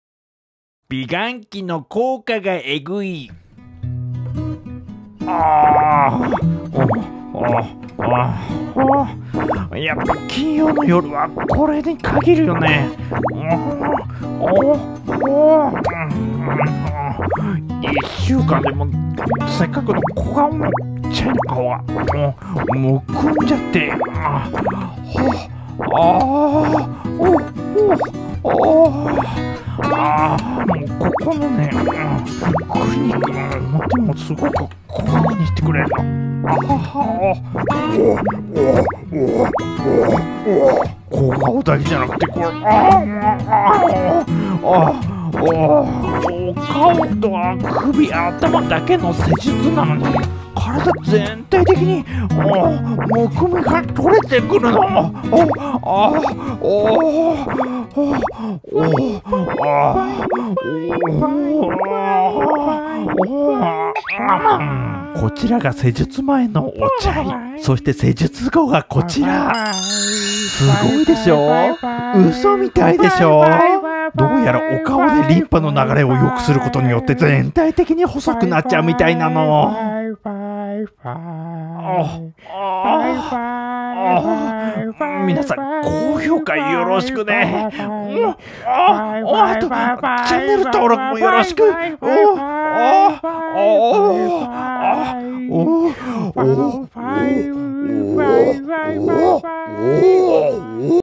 美顔器の効果がエグい
ちゃい（三毛猫）は金曜の夜はマッサージが定番なの。とってもおすすめのプレミアム美顔器を使ったフェイスマッサージの効果をご覧くださいだよね♪

moshinukoとは...